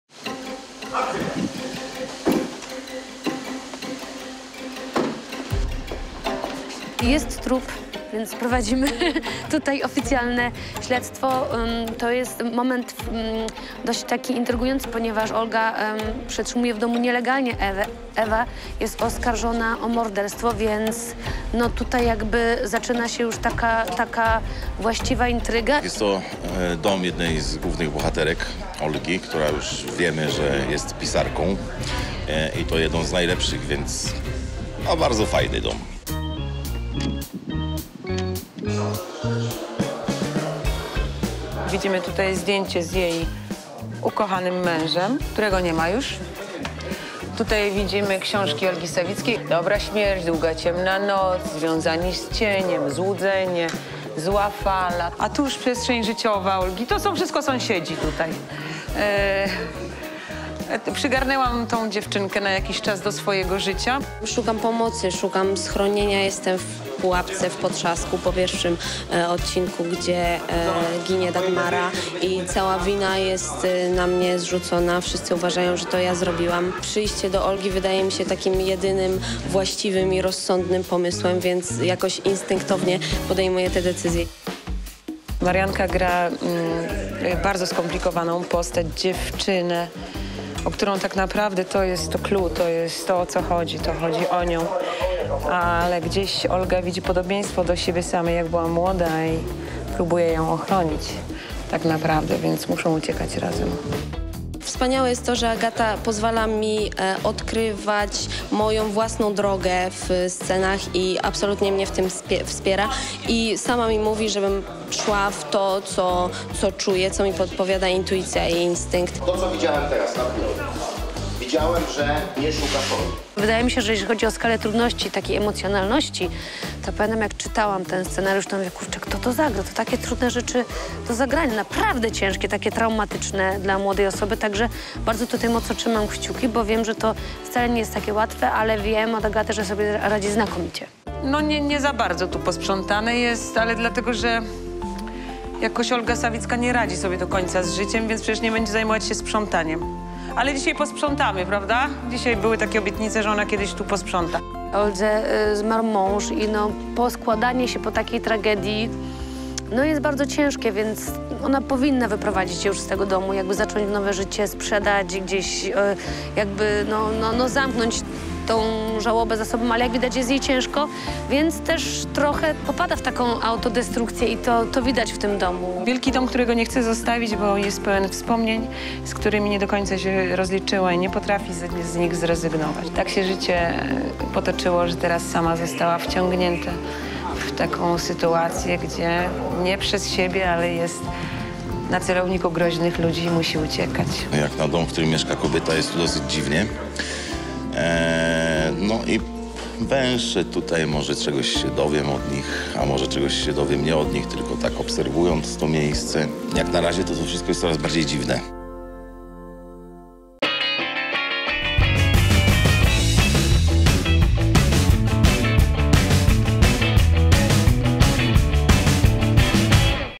Jak mieszka Olga Sawicka? Agata Kulesza oprowadziła nas po luksusowej willi!
Oglądaj "Pułapkę" już w niedzielę o 21:30 w TVN oraz w Player.pl
Byliśmy na planie sesji zdjęciowej bohaterów serialu "Pułapka". Aktorzy między ujęciami opowiedzieli nam o kulisach serialu. Co skłoniło Leszka Lichotę do wzięcia udziału w serialu? Agata Kulesza przyznała, że sceny na planie są dla aktorów bardzo wymagające. Co to znaczy? Marianna Kowalewska, odtwórczyni roli Ewy dodała, że aktorzy choć mają większe zawodowe doświadczenie niż ona, traktują ją po partnersku. Czy Agata Kulesza udział jej aktorskich porad?